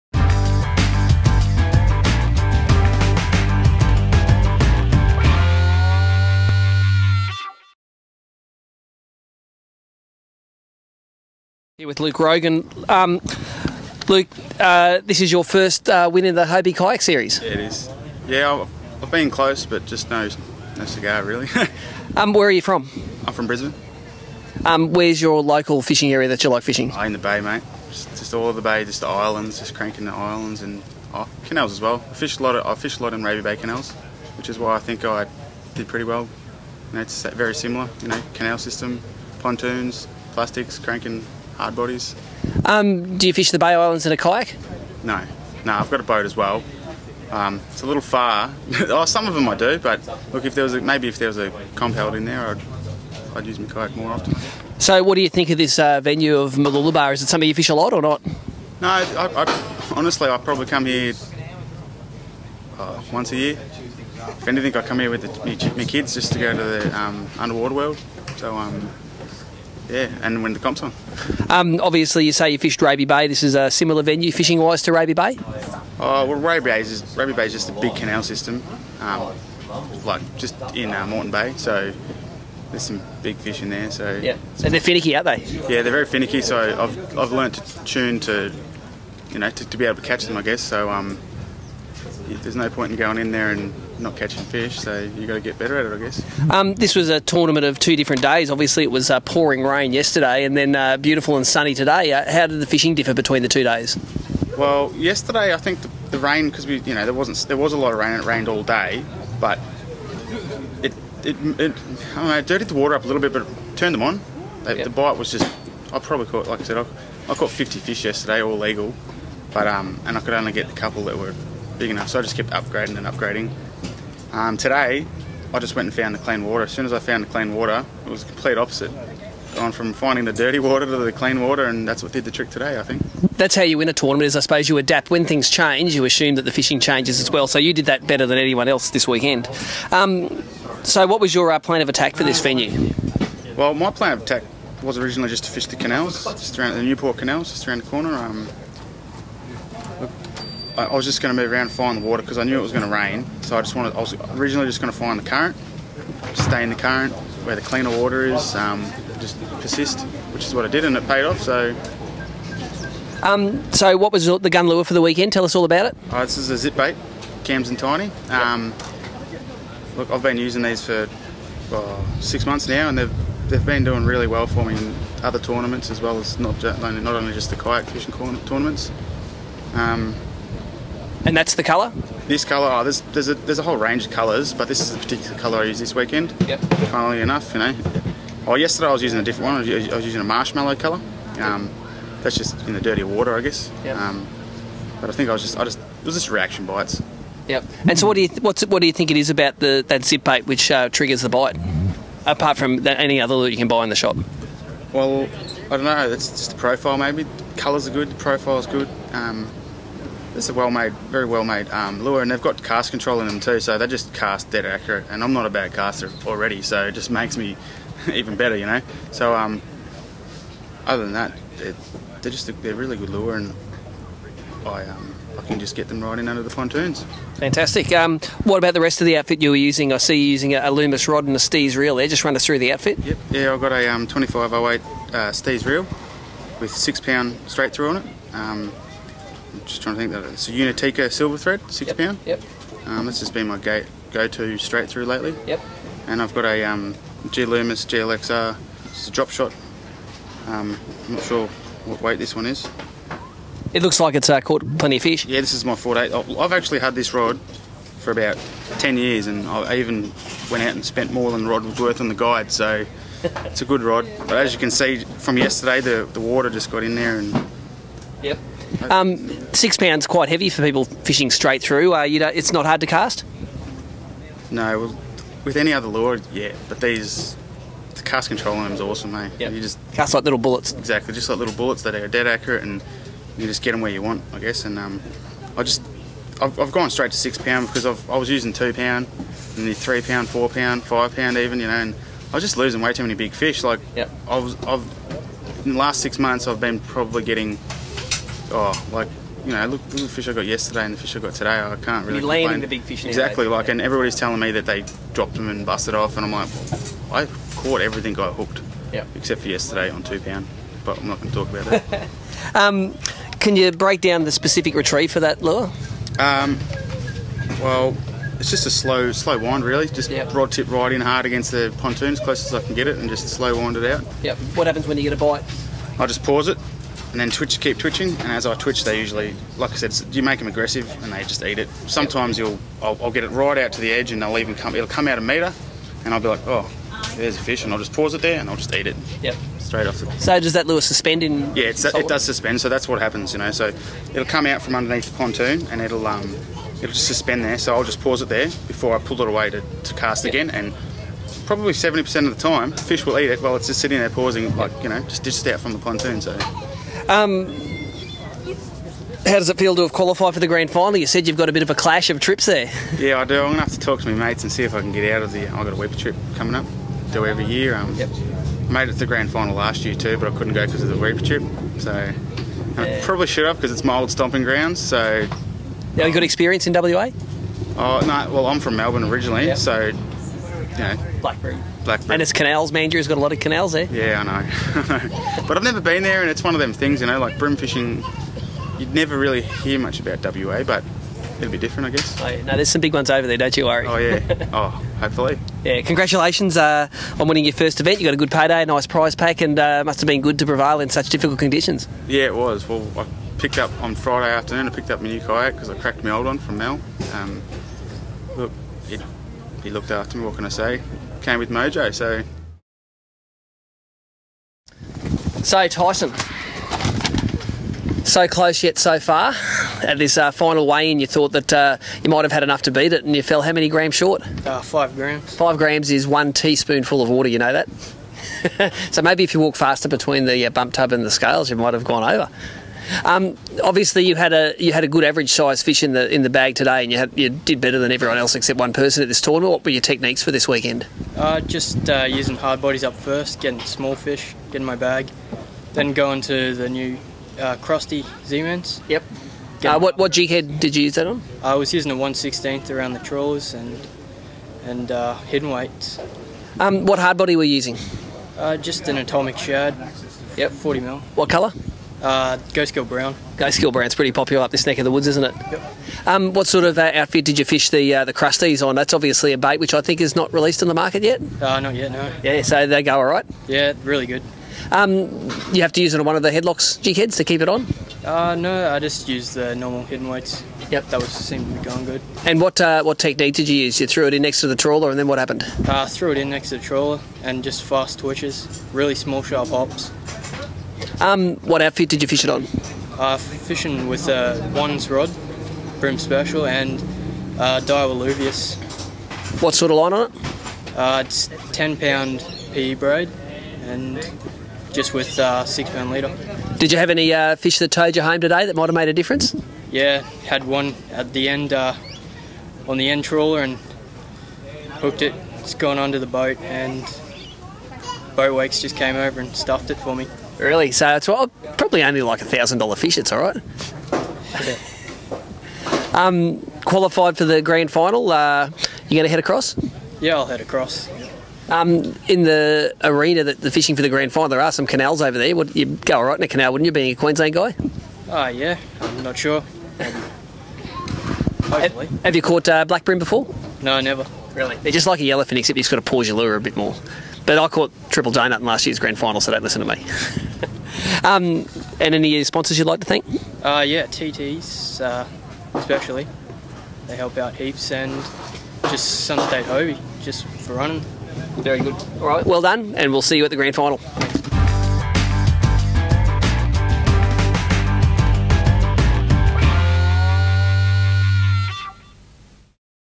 Hobie Mooloolaba Event Winner 2014
Luke Rogan and Tyson Hayes explain their successful techniques from the Mooloolaba Round of the Hobie BREAM Sereis. They were separated by just 5 grams at the end of 2 days fishing.